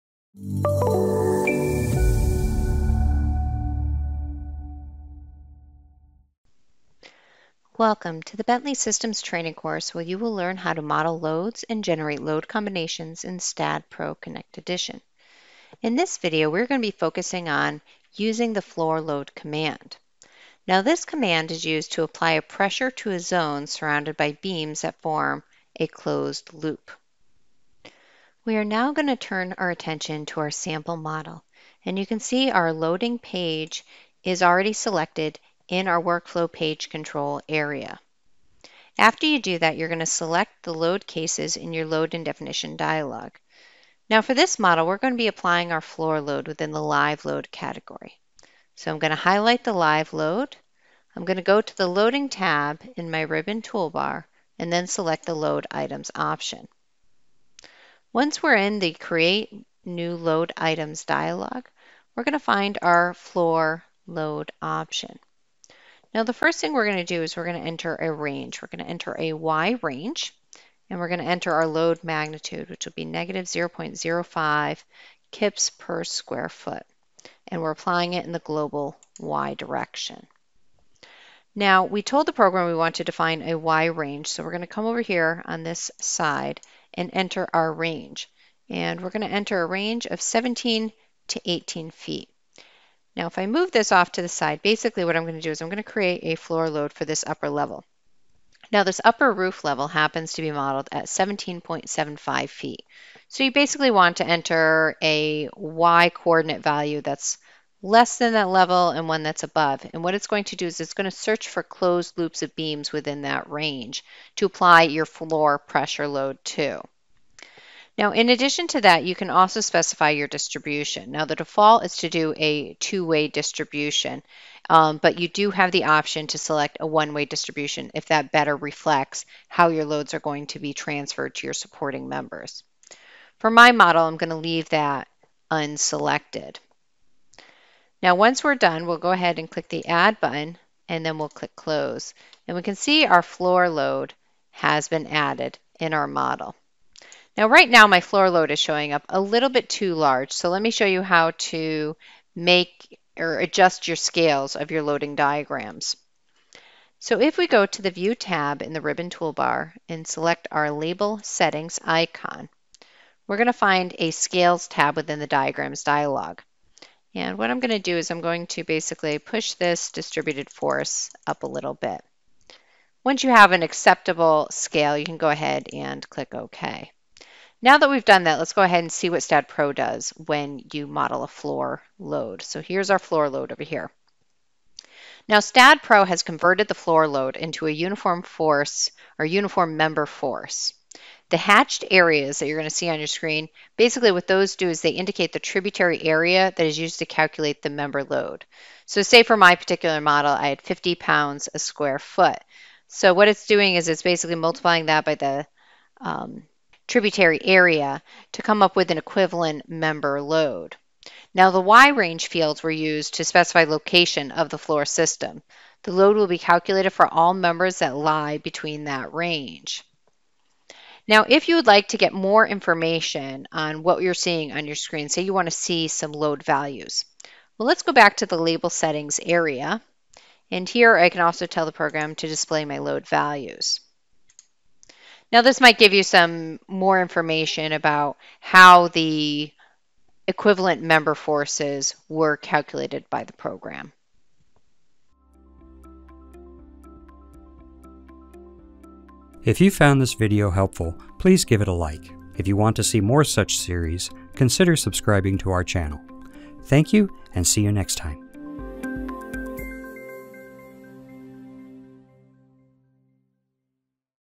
Modeling Floor Loads in the STAAD.Pro Analytical Modeler
In this video, you will learn how to model floor loads in the STAAD.Pro Analytical Modeler.